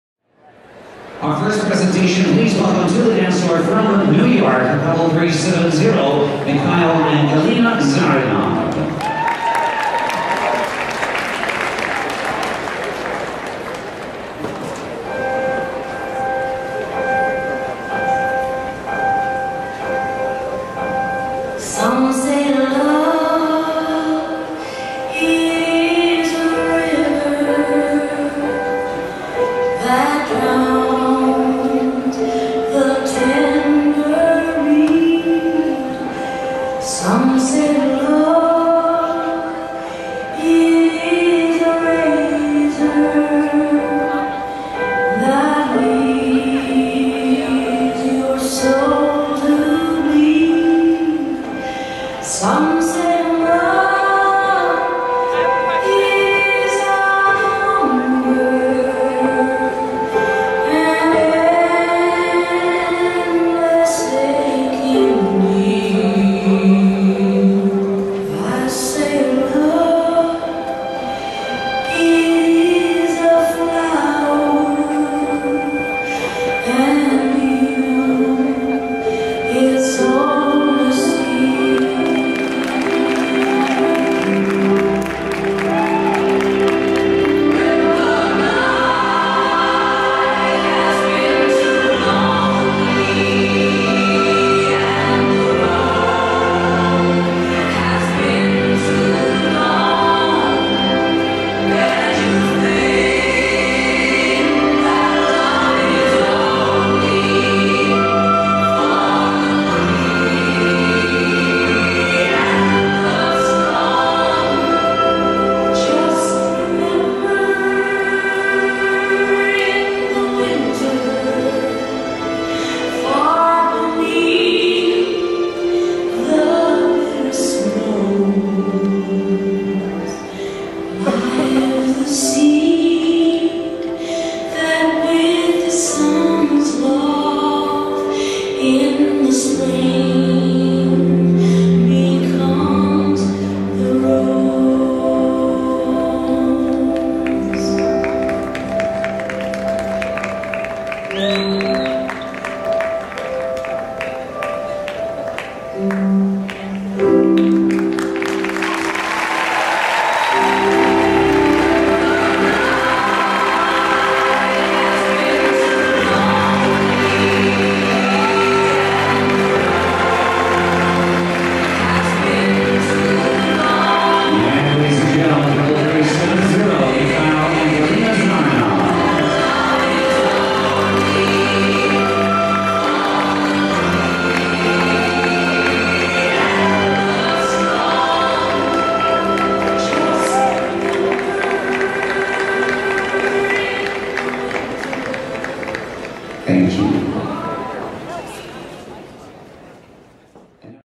2012 Ohio Star Ball Mikhail and Galina Zharinov Showdance
Ohio Star Ball is one of the premier Ballroom & Latin dance competitions in the world. Takes place every November in Columbus, Ohio.